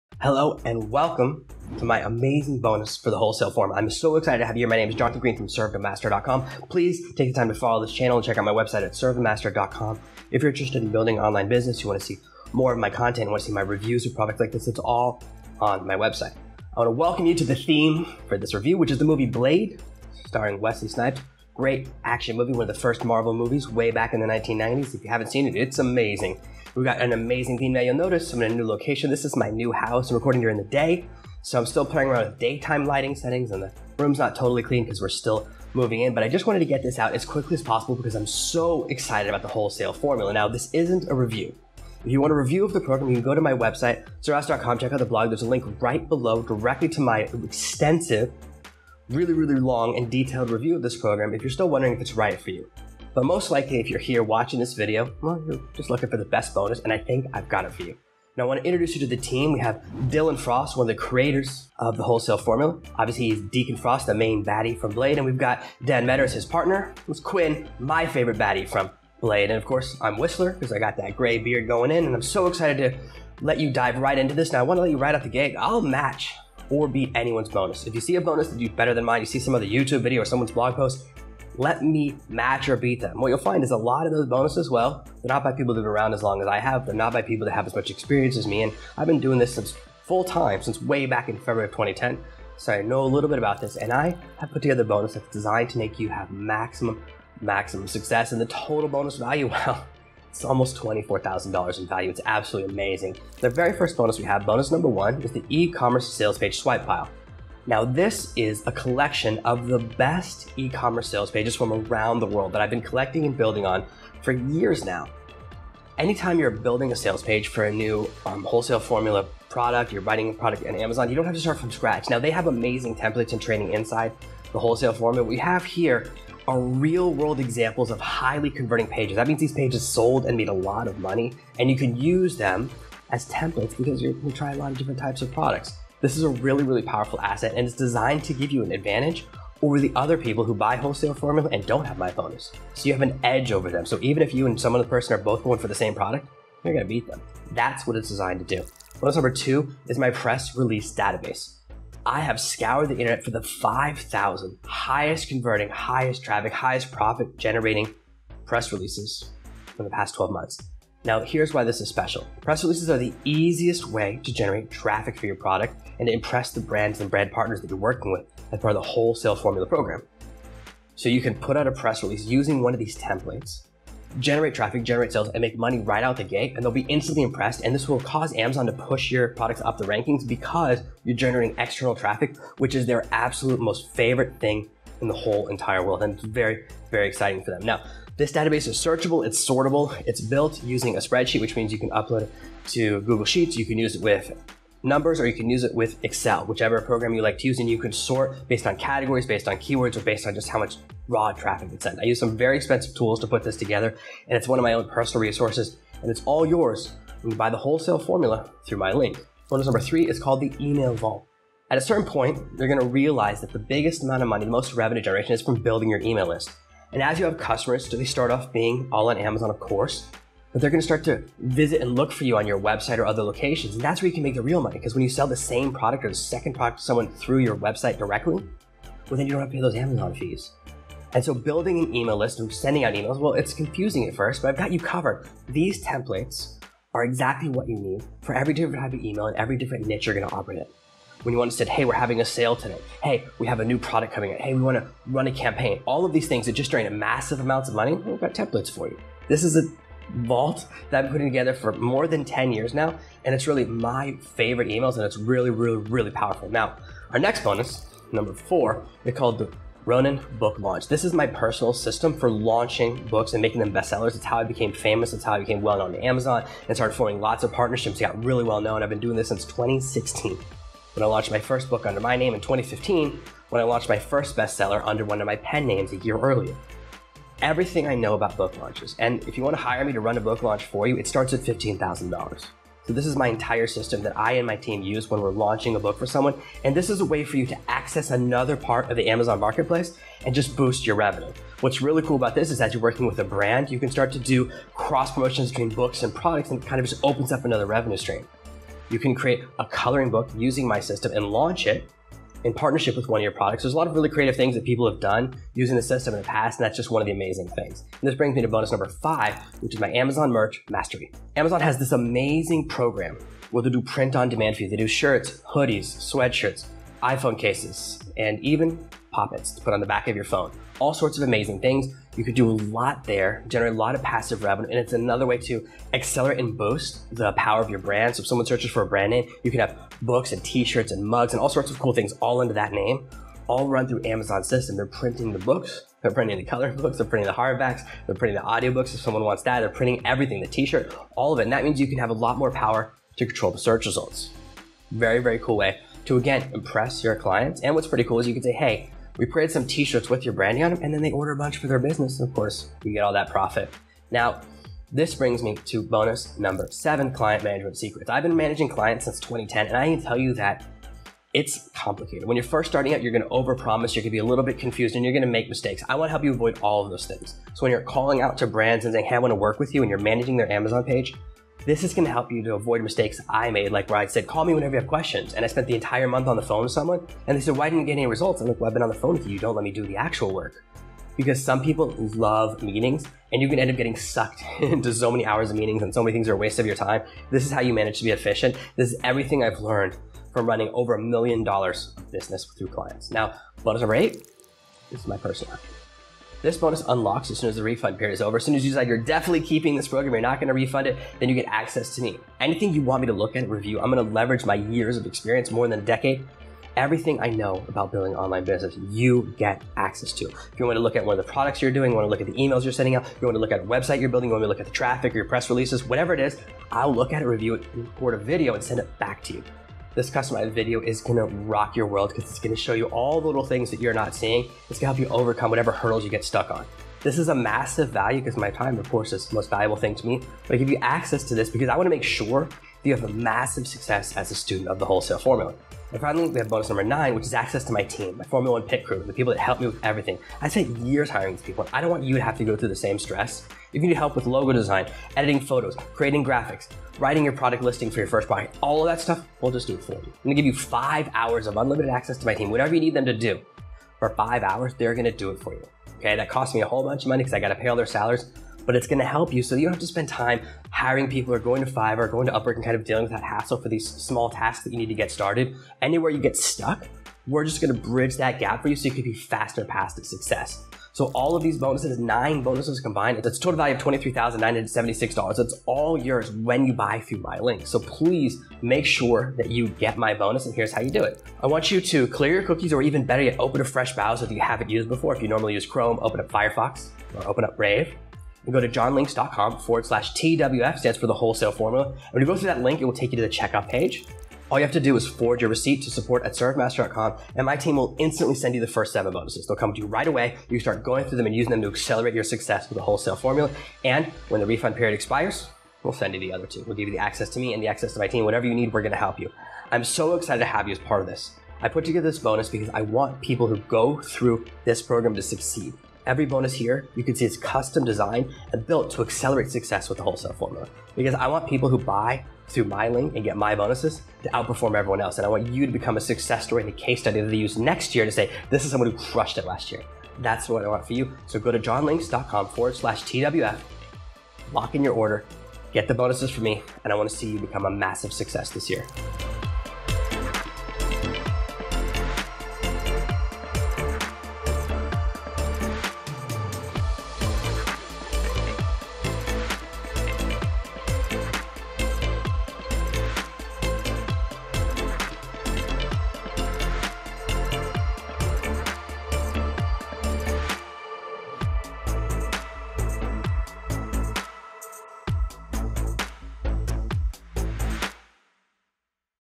TheWholesaleFormulaBonus2022
Welcome to my 2022 bonus for The Wholesale Formula by Dylan Frost and Dan Meadors. If you’re looking for my wholesale formula review, that’s in the blog post linked below.  I’ve put together a bonus to supplement this legit course and help you to succeed with this amazing program.

[Bonus 6 = Amazon Ads Unlocked]